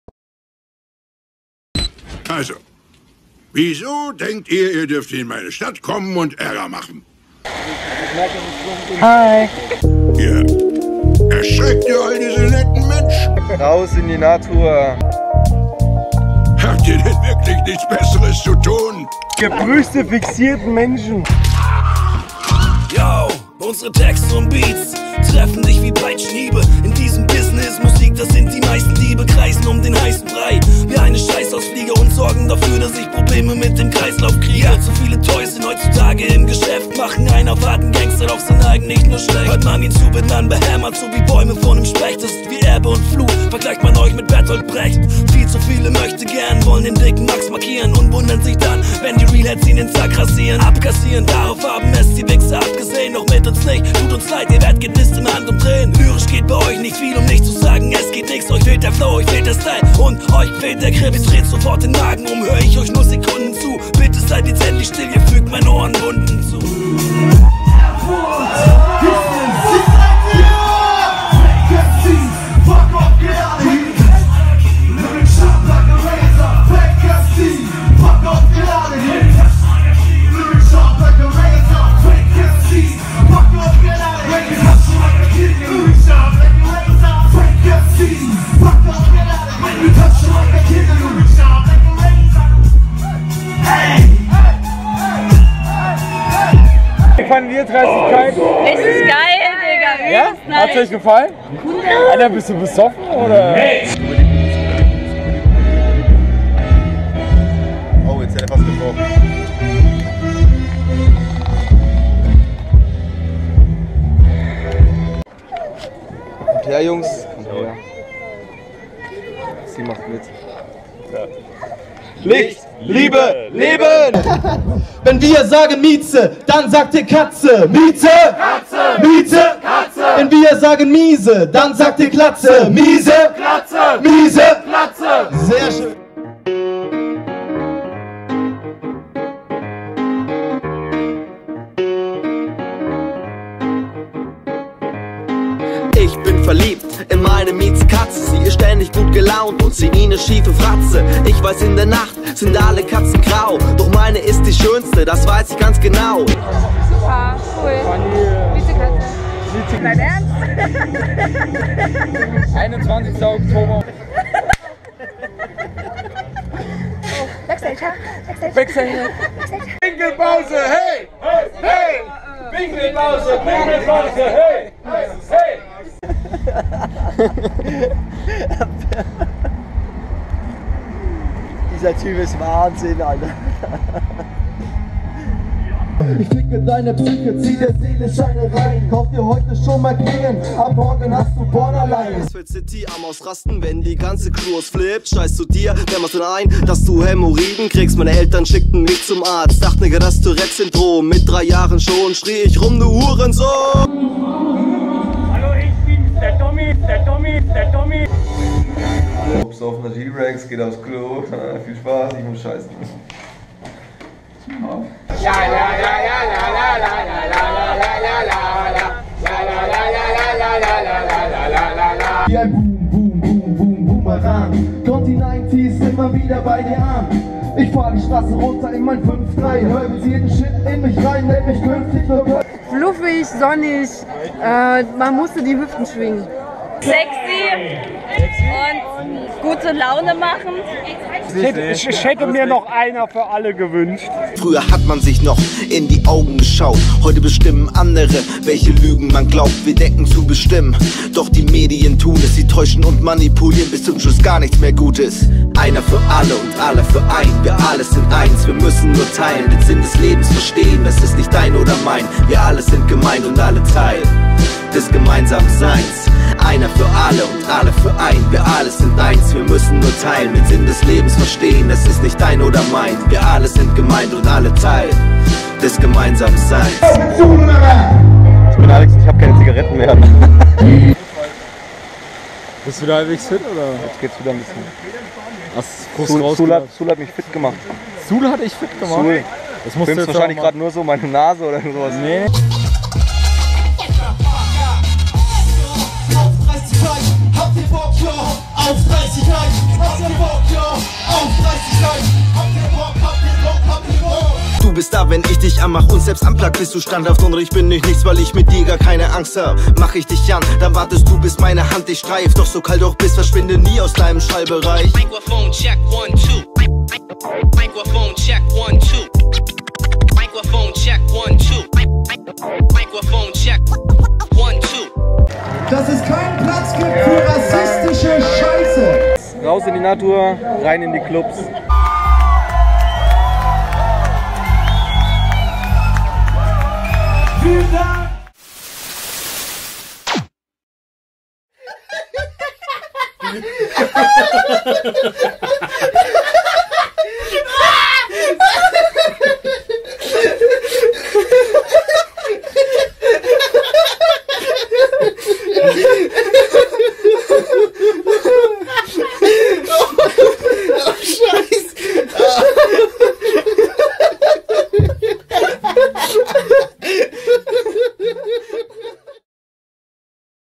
Raus in die NaTour 2017 I AFTERMOVIE
Aftermovie by: Blinz

Danke an alle die mit uns 2017 "Raus in die NaTour" gegangen sind !

Danke an alle die uns geholfen und supportet haben !

Danke an alle Veranstalter und Orga-Teams !

Danke an alle Bands mit denen wir auf der Bühne stehen durften !

Licht - Liebe - Leben